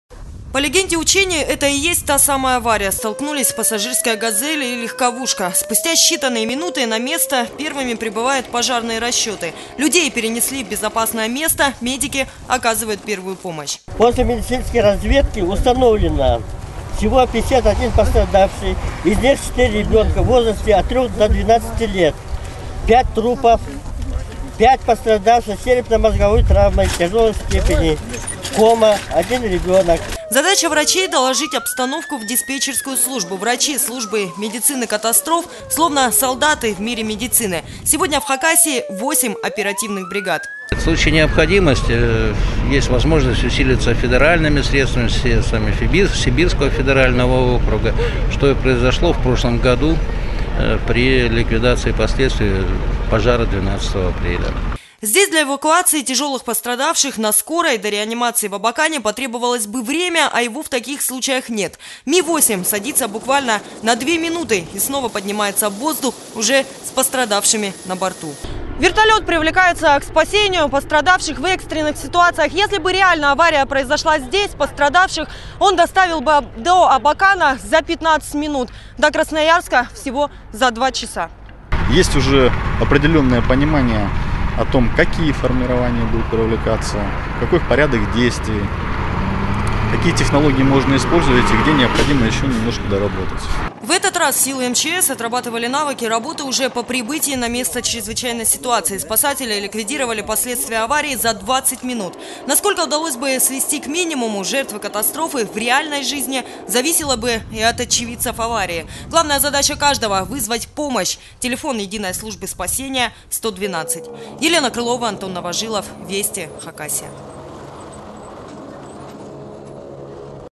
В Хакасии спасатели провели масштабные учения по ликвидации крупных автокатастроф 24.08.2016
Все решают минуты! За 20 минут ликвидировали последствия условной крупной аварии на трассе Абакан-Саяногорск спасатели. Прошли масштабные учения всех подразделений. Привлекались пожарные расчеты, вертолет МИ-8, силы центра медицины катастроф и сотрудники ГИБДД. Как в команде работают оперативные службы?